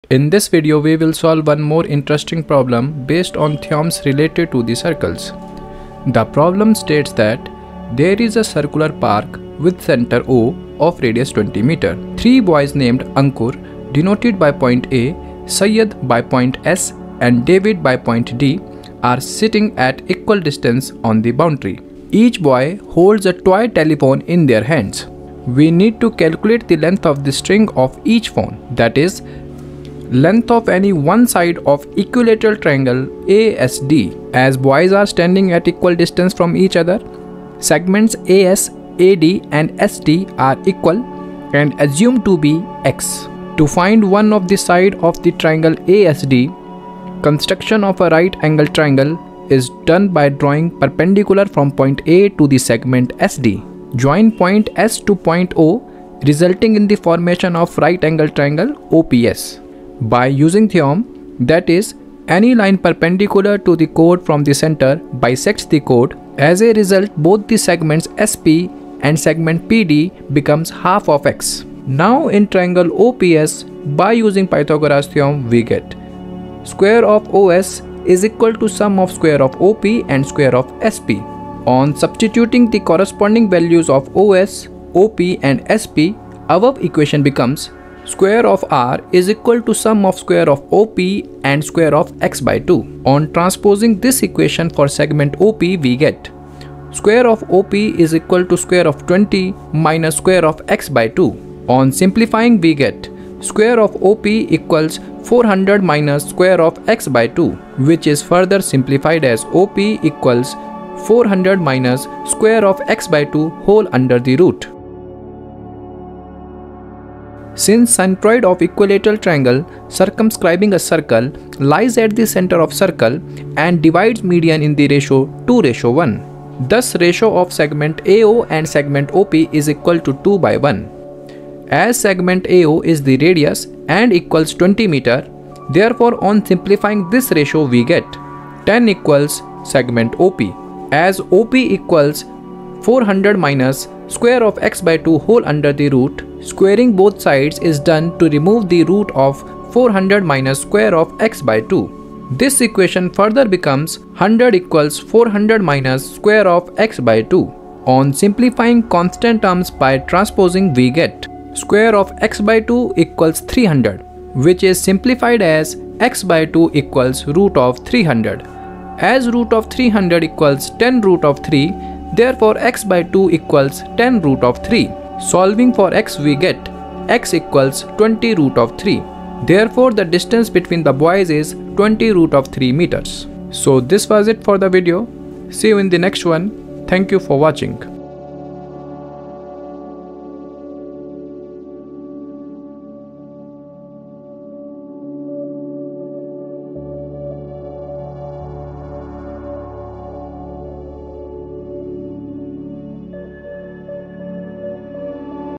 A circular park of radius 20m is situated in a colony. Three boys ..............|Ex-10.4|NCERT.
This video explains how to solve problems related to the circle. the problem states,
"A circular park of radius 20m is situated in a colony. Three boys Ankur ,Syed and David are sitting at equal distance on its boundary each having a toy telephone in his hands to talk to each other. Find the length of string of each phone."

The video is a detailed explanation and with visual representation. More videos on the way.